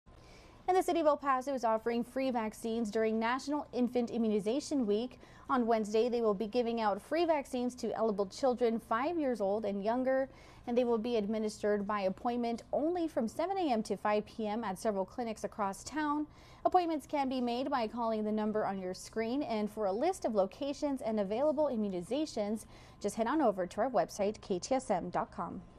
City of El Paso offers free vaccines to children.
City of El Paso offers free vaccines to children during National Infant Immunization week by appointments only.